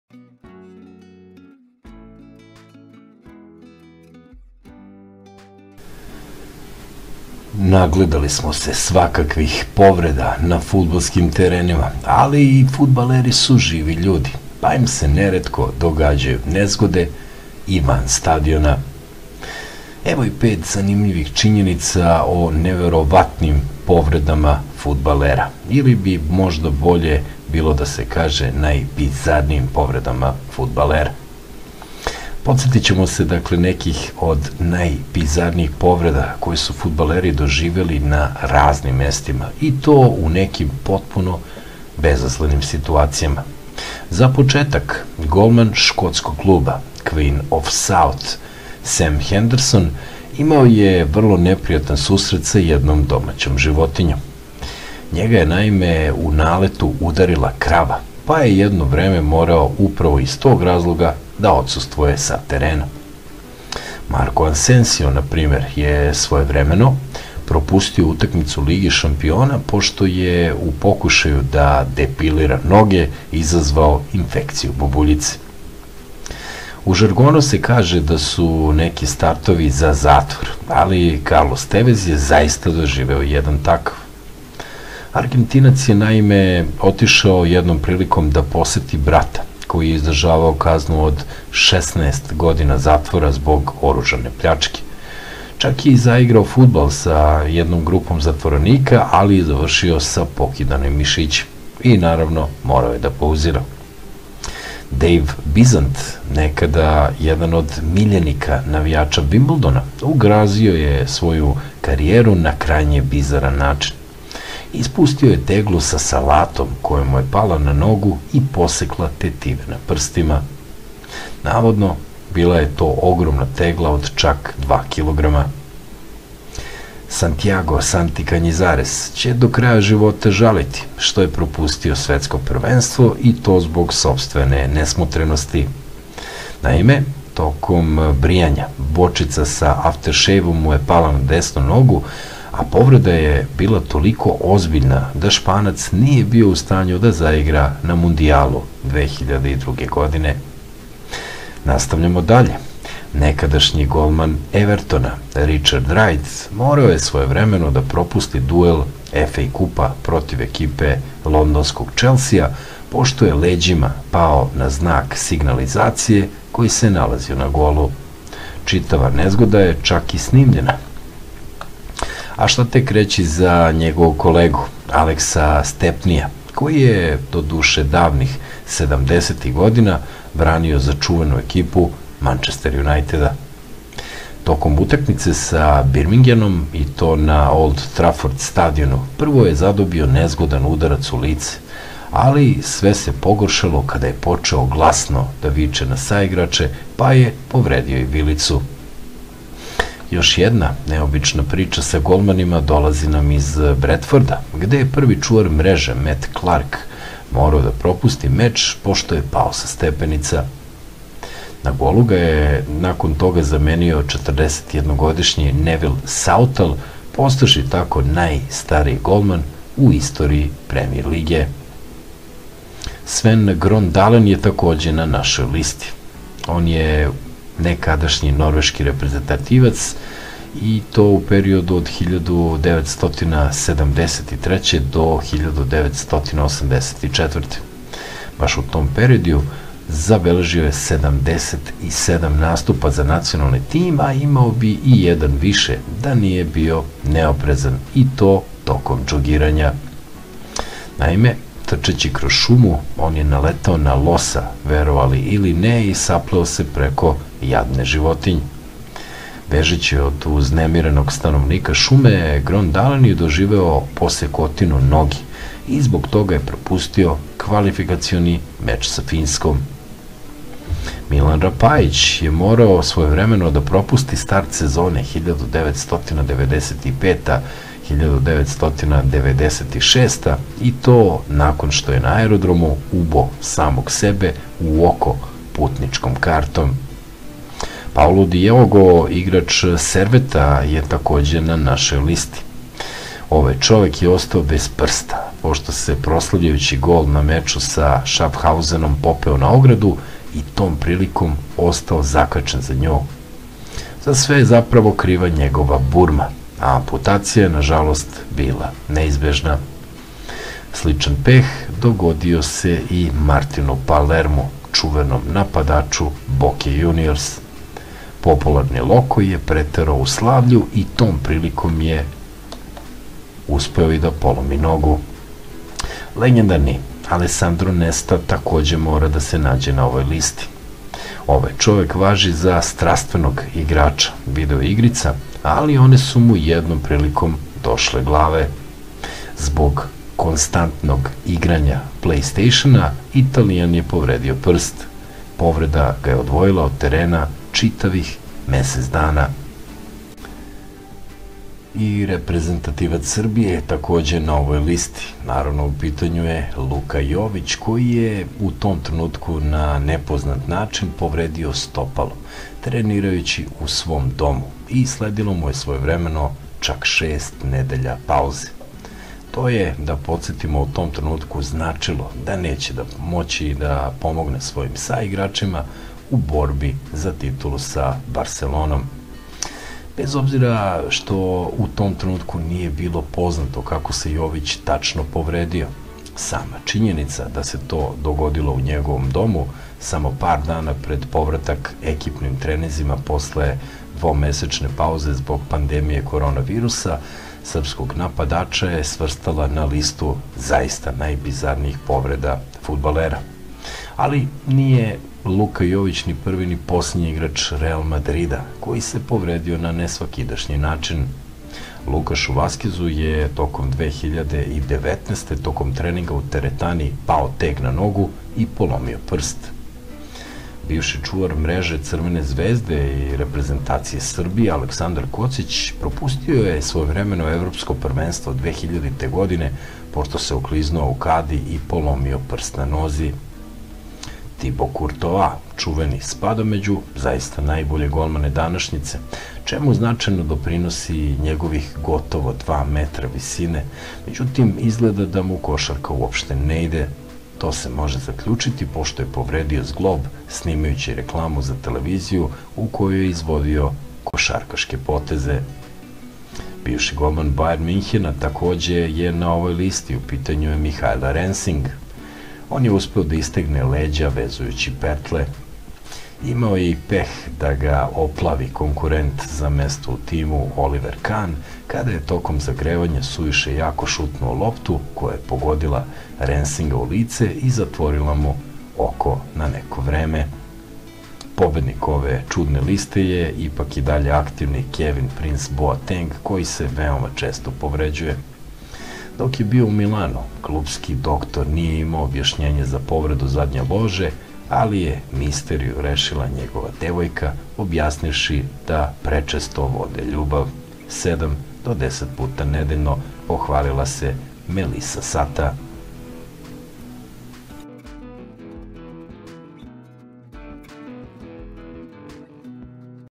Najbizarnije Povrede u Istoriji Fudbala
ima, ipak, mnogo luđih slučajeva, pa evo nekih od njih:

GOLMANI I UKLETA KUPATILA

Bivši čuvar mreže Crvene zvezde i naše reprezentacije, Aleksandar Kocić, propustio je Evropsko prvenstvo 2000. pošto se okliznuo u kadi i polomio prst na nozi, dok je Španac Santijago Kanjizares izostao sa Svetskog prvenstva 2002. jer je pokušao da "uštopuje" losion za posle brijanja. Nije uspeo, a parče stakla iseklo mu je tetivu palca...

ŠTOPERI I BAKSUZNI "PES"

Video-igre su jedna od omiljenih zanimacija fudbalera, ali Alesandro Nesta i Rio Ferdinand doživeli su vrlo neprijatno iskustvo igrajući popularnu simulaciju fudbala "PES". Italijanski štoper je istegao zglob ruke predugim igranjem svog "Sonija", dok je njegov kolega iz Engleske, uživajući u "PES"-u pred svojim televizorom povredio koleno tako što je predugo držao nogu u ispruženom položaju, a zatim pokušao da se protegne i stopalom dohvati telefon.

Srećom po njega, bilo je to pre nego što je iz Lidsa prešao u Mančester junajted, jer ko zna šta bi mu rekao ser Aleks Ferguson. Ili Nemanja Vidić i Roj Kin.

(U vezi sa Ferdinandovom povredom postoji i verzija po kojoj je gledao TV, predugo držao nogu u blago podignutom položaju, pa istegao ligamente kada je pokušao da dohvati šolju kafe)

REALOVI MALERI

Nije Luka Jović ni prvi ni poslednji igrač Real Madrida koji se povredio na nesvakidašnji način. Lukasu Vaskezu je prošle godine tokom treninga u teretani ispao teg na nogu i polomio prst, dok je MarkoAsensio zaradio infekciju posle brijanja nogu.

A, SAMO SU HTELI NEŠTO DA PREZALOGAJE

Nekadašnji defanzivac Glazgov Rendžersa, Kirk Brodfut, mogao je ozbiljno da nastrada kada mu je u lice eksplodiralo jaje koje je želeo da zagreje u mikrotalasnoj. Zbog opekotina je morao u bolnicu, a svakako u tom periodu nije mogao da nastupa, jer je imao povredu zgloba.

Povredu u kuhinji doživeo je i bivši engleski golman, Dejv Bizent, na način identičan Kanjizaresovom,s tim da je on pokušao da "uštopuje" staklenu činiju za salatu tešku dva kilograma koja mu je ispalada dok je spremao sendvič"!

JESTE VISOK, ALI NIJE ZA KOŠARKU

Tibo Kurtoa spada među najbolje golmane današnjice, čemu značajno doprinosi njegovih skoro dva metra visine, međutim, izgleda da mu košarka uopšte ne ide. To se može zaključiti pošto je povredio zglob snimajući reklamu za TV u kojoj je izvodio košarkaške poteze.

ODMOR MU BAŠ NE PRIJA

Golmani su, izgleda, "majstori" da se povrede van terena, pa je na ovoj listi i Dejvid Džejms. Nekadašnji reprezentativac Engleske i dugogodišnji čuvar mreže u Liverpulu tu je zbog dve situacije. Jedna je poput Ferdinandovog leškarenja, kada je gledajući TV istegao leđa pokušavajući da dohvati daljinski upravljač, a druga je sa pecanja, kada je iščašio rame pokušavajući da izvuče povećeg šarana.

Izgleda da mu ne prija ono što drugi ljudi rade kada žele da odmore...

SAIGRAČI SU KRIVI

Nastavljamo dalje sa momcima među stativama. Na ovu listu smo prosto morali da uvrstimo nekadašnjeg golmana Mančester junajteda, Aleksa Stepnija, koji se toliko drao na saigrače tokom utakmice protiv Birmingema da je iščašio vilicu! Bilo je to 1975. godine.

POPUSTI GAS, POVUCI RUČNU

Bivši bek Aston Vile, Alan Rajt, toliko je bio nizak da bi dovoljno pritisao gas svog novog Ferarija, da je uspeo da istegne ligamente kolena tokom vožnje.

Argentinac Ever Banega nema taj problem, ali je uspeo da bude pregažen od svog automobila, pošto je zaboravio da podigne ručnu kočnicu kada je stao da natoči gorivo. Stradalo je stopalo...

GLUMIO GOLMANA

Brazilac Emerson je propustio Svetsko prvenstvo 2002. u Japanu i Južnoj Koreji pošto je tokom jednog treninga svoje reprezentacije rešio da se oproba kao golman i iščašio rame.

KOBNE PROSLAVE

Pojedinima na ovoj listi presele su proslave golova: Martinu Palermu i Stivu Morovu.

Poznati Argentinac je igrajući za Viljareal slavio pogodak sa navijačima, ali su oni pod pritiskom oborili reklamu koja je pala na njega i slomila mu nogu, dok je bivši igrač Arsenala pao sa ramena saigrača Tonija Adamsa, polomivši ključnu kost.